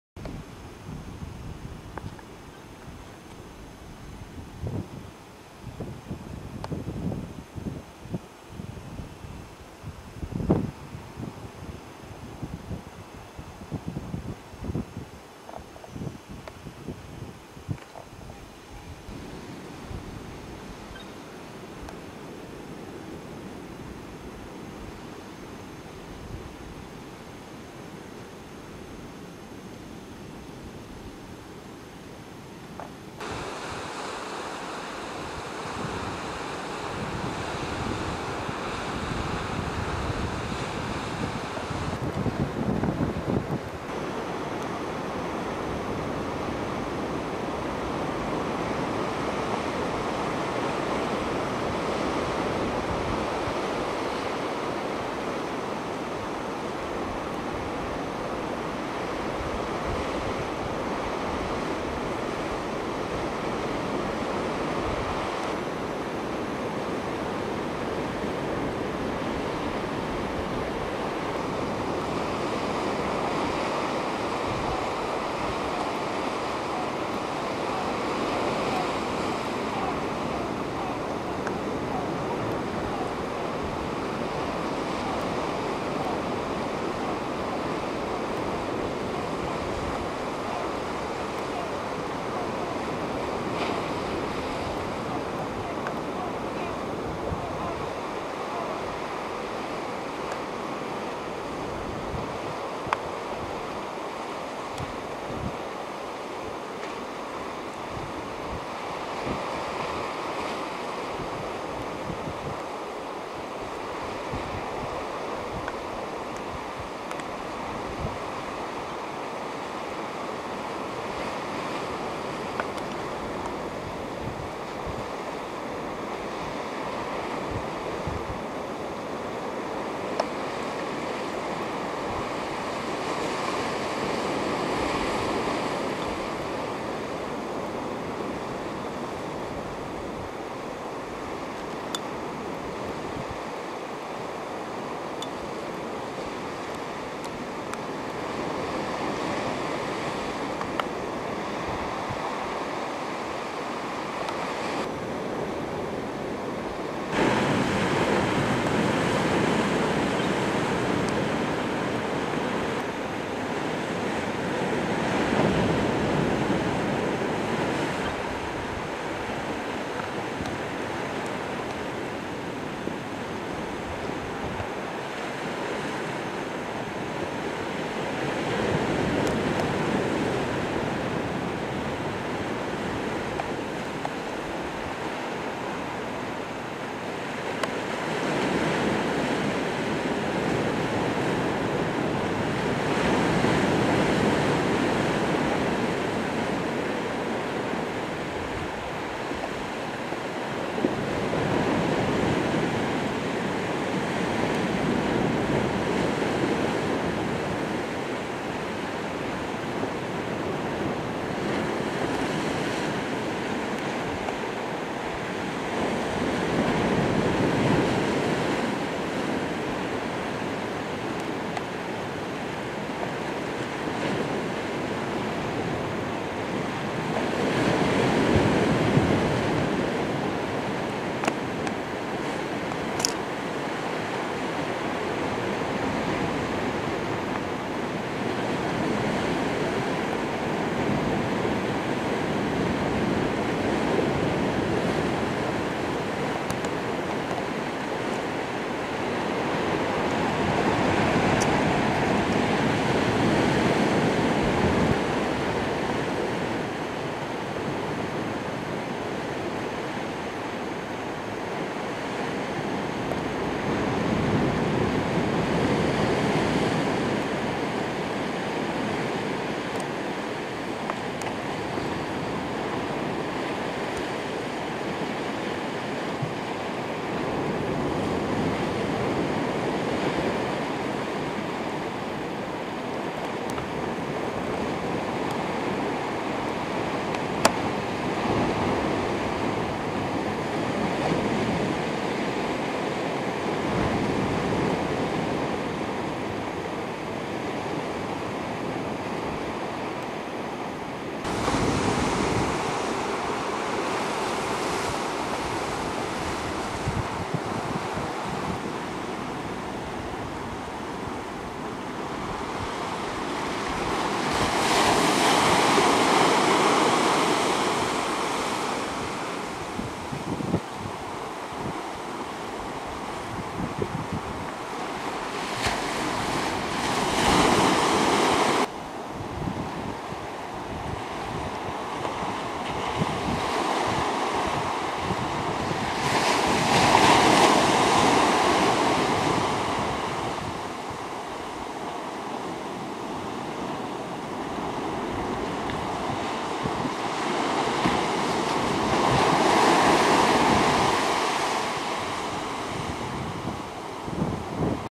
M/V"Cap Finistère dans le Froñ Veur.wmv
Le Ferry "Cap Finistere"  de la Brittany Ferries...dans le Froñ Veur ( entre Molene et Ouessant)
le 25 novembre 2011 vers midi